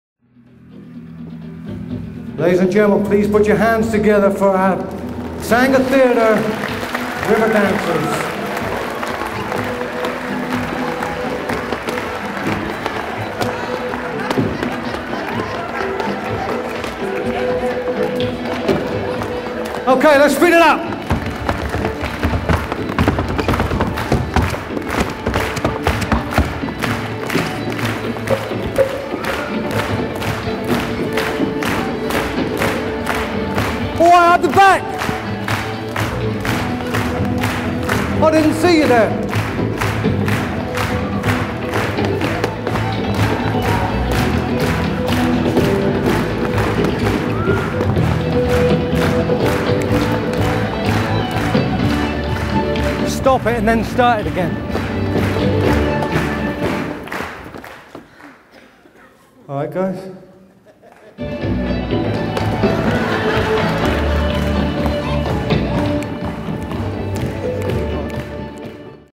Lee Maskery Comedy Stage Hypnotist / River dance
These people think they are all River Dancers, Ha Ha